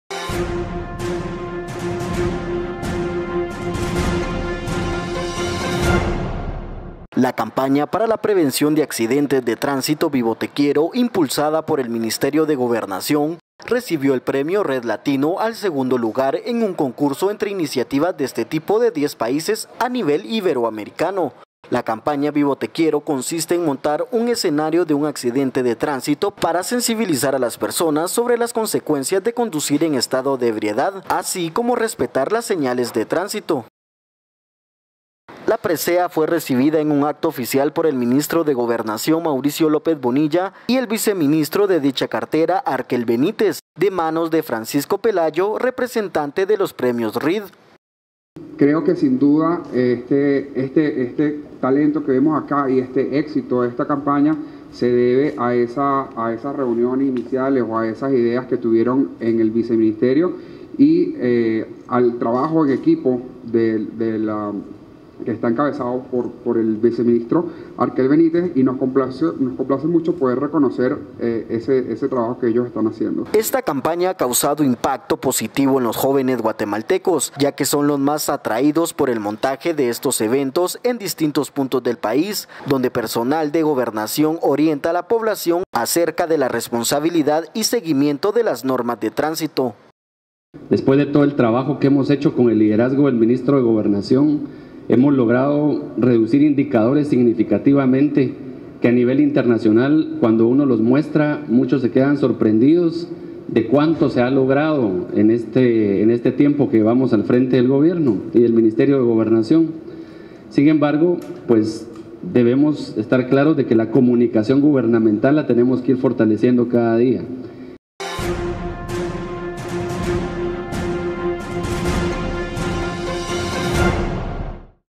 Gobernación recibe premio Reed Latino por campaña preventiva
Por Oliver Paniagua/DCA. Por concientizar en la prevención de accidentes de tránsito, la campaña ¡Vivo te Quiero! del Ministerio de Gobernación, recibió el segundo lugar en el certamen Reed Latino entre propuestas de 10 países. Es la primera vez que el galardón se entrega en Latinoamérica.